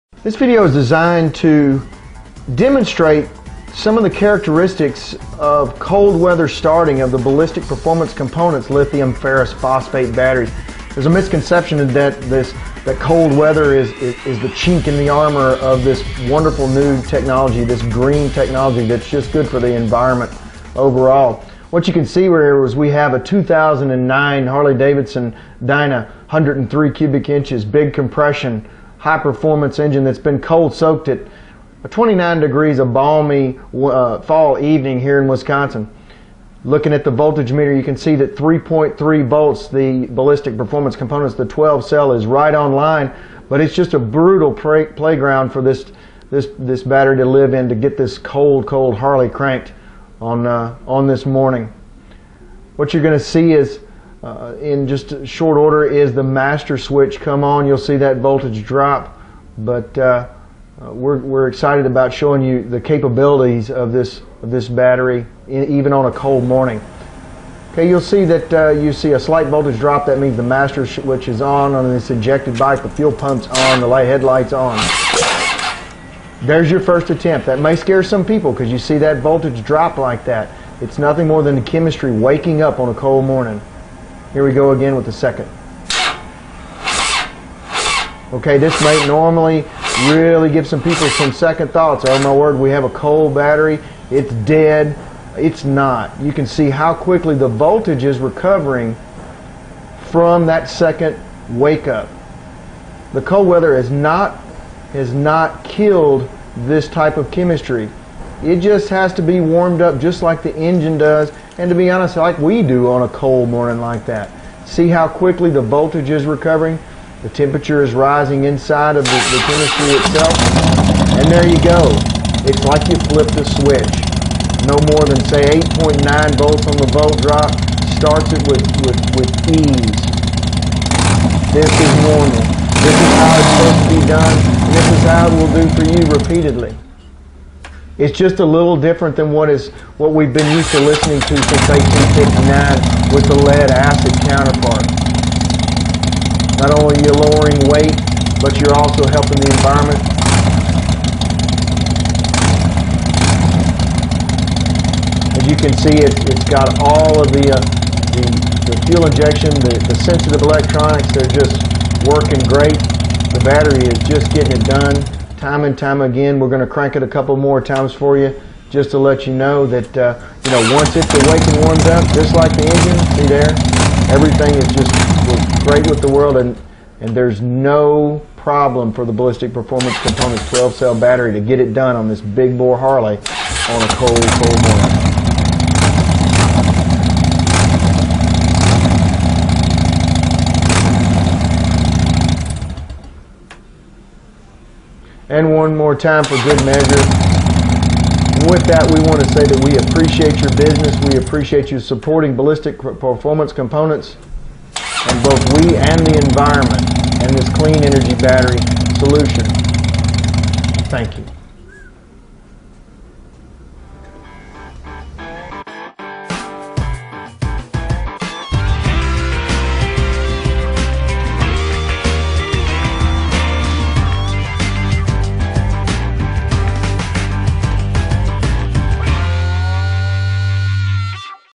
Ballistic EVO2 Battery & The Cold Starting of LiFePO4 Batteries - on Motorcycle-Superstore.com TV
Your powersports vehicle is the pinnacle of modern technology. It is made with the latest lightweight, high tech materials to be the most high performance vehicle on the road, lake, track or trail. You have modified your powersports vehicle with the best in carbon fiber, titanium, and magnesium in an effort to gain every last ounce of performance. Yet, under your seat is a heavy hunk of lead that was designed in the mid 1800's.

Ballistic Performance Component Batteries use the latest, state of the art lithium ferrous energy storage technology available to provide you with a smaller, higher energy, lightweight motorcycle battery. Each Ballistic Performance Component Battery is assembled by hand in the USA using computer designed custom proprietary cells, cases, and hardware. As a result, Ballistic Performance Component Batteries will provide more energy out of a smaller package while requiring almost no regular maintenance and under most conditions a longer service life than existing lead acid batteries. One of the best ways to

Features:

-Up to 80% smaller than the stock battery
-Up to 10lbs Lighter than the stock battery
-Service life of over twice a lead acid in similar conditions
-Industry leading 3 year warranty
-Designed, developed, and assembled in the USA
-Hard mounted brass terminals for a direct replacement installation
-Completely "dry" technology so batteries can be mounted in any direction
-Non-toxic, and recyclable
-Applications for powersports vehicles from 50cc - 1800cc
-Tested extensively by professional race teams
-Impact and water resistant
-No special charger required
-No trickle charger required, loses only 10% of charge over a year of static use
-These batteries operate very differently than lead acid type, please watch the video for more details